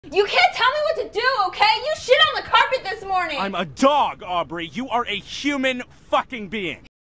10 Word Lip Sync - Rough
Lip syncs are so much fun. Definitely my favorite thing to animate. Muzzle syncs are even more fun for me!

This is just the rough pass of my assignment, which was to animate a lip sync to an audio clip of no more than 10 words. It will be fully animated by next week.

((Also heads up, has some foul language.)